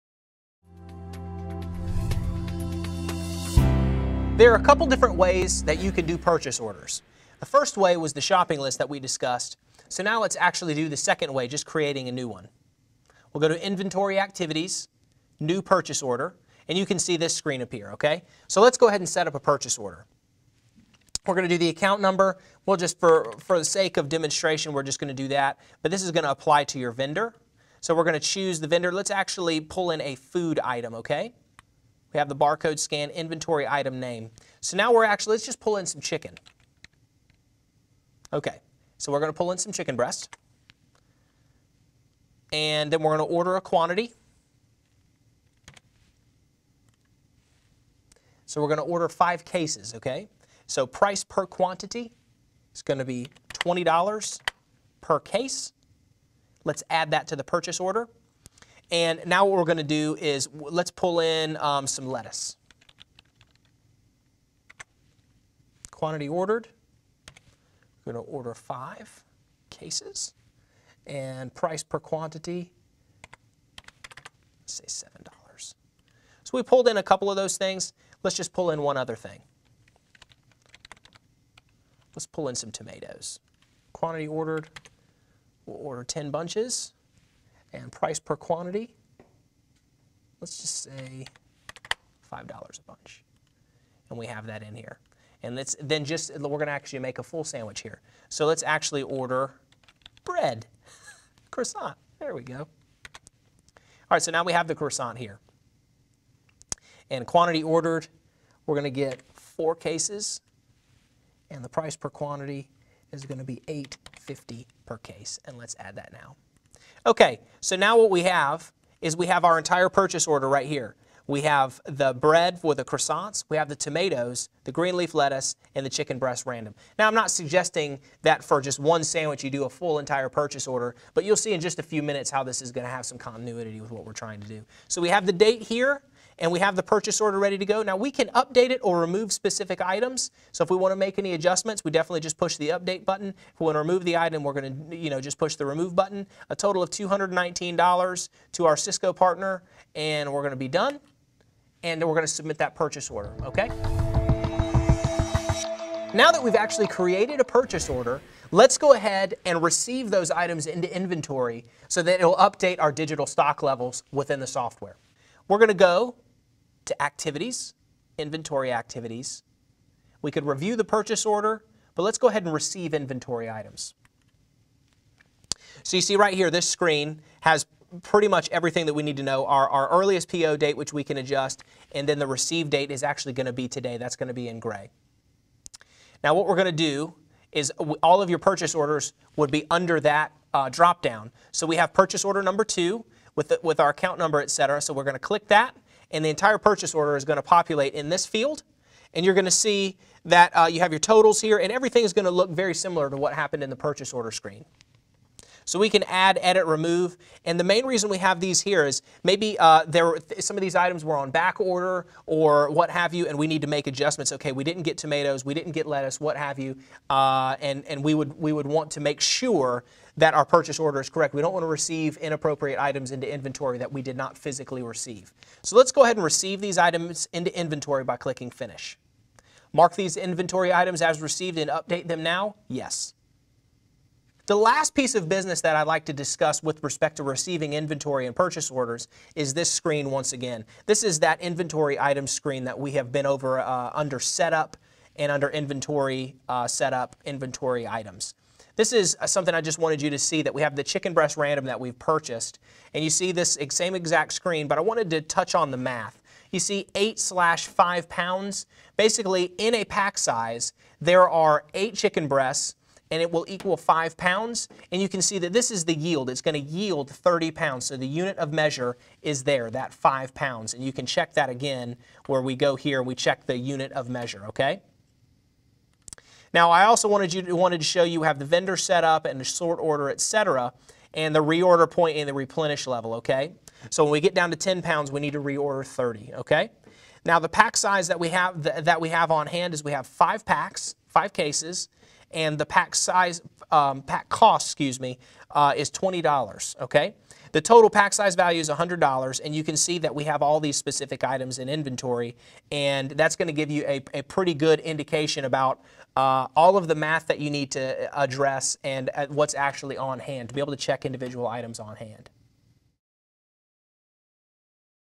Aldelo - Creating Purchase Orders
00:02 - Creating Purchase Orders (PO's)
02:36 - Receiving Inventory